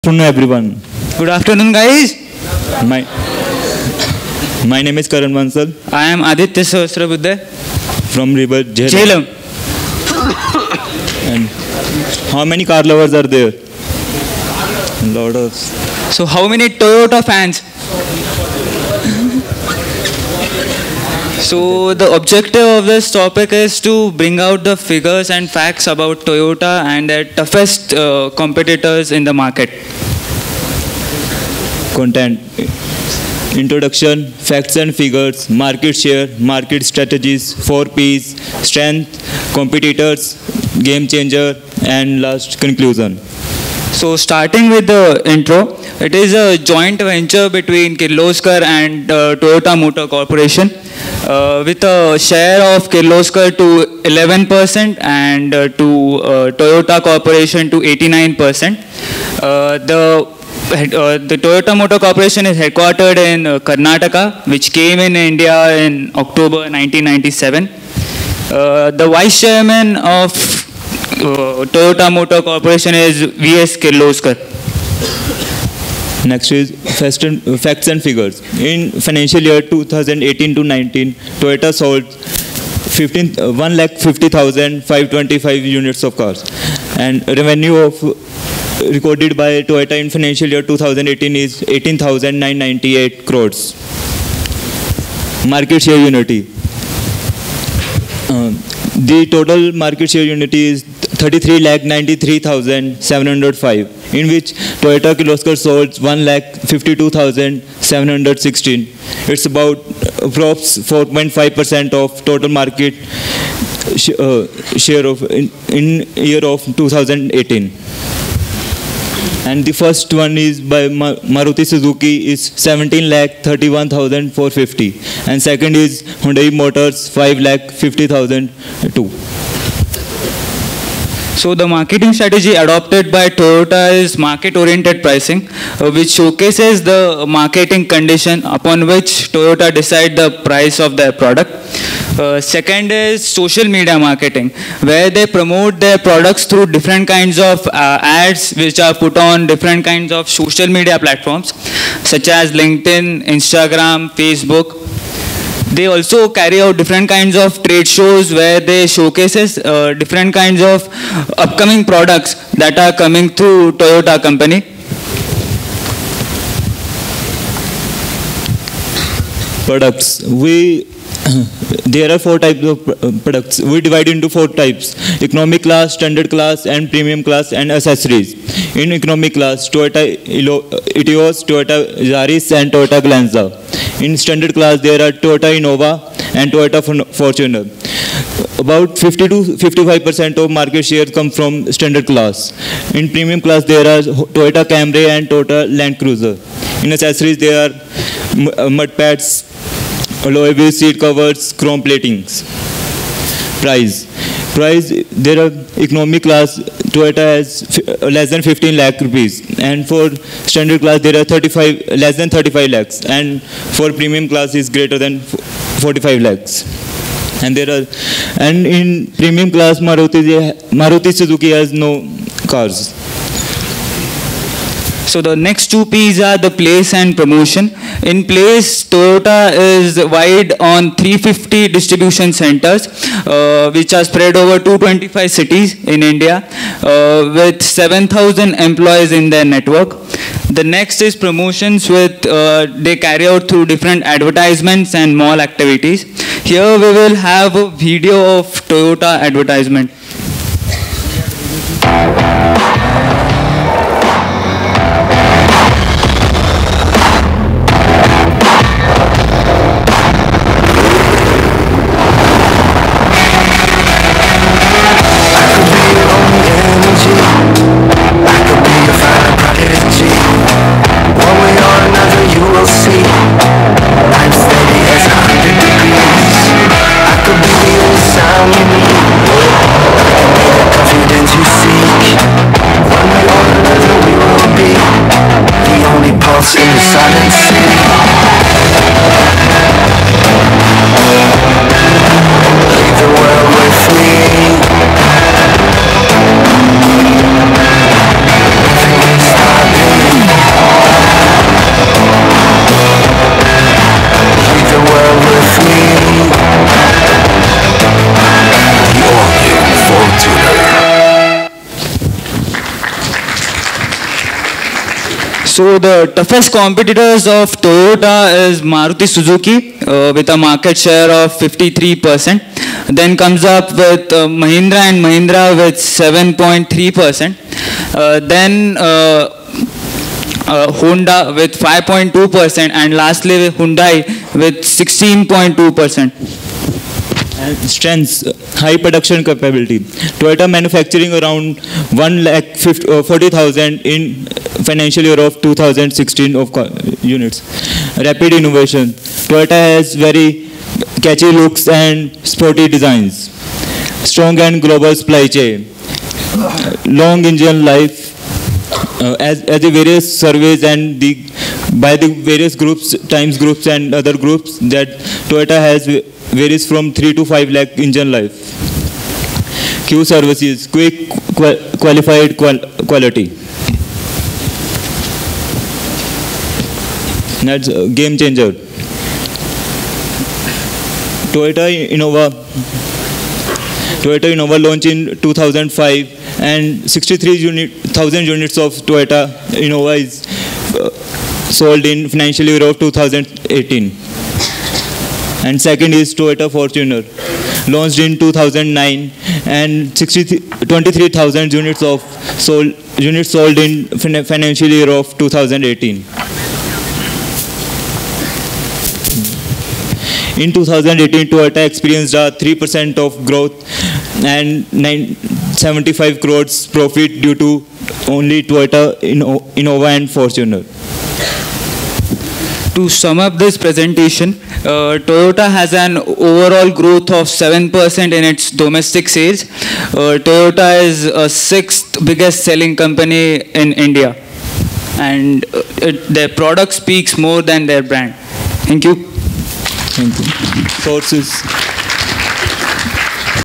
Sri Balaji Society Induction Presentation 2019 - A Strategy Study about Toyota Motor
TOPIC -   A Strategy Study about Toyota motor corporation in India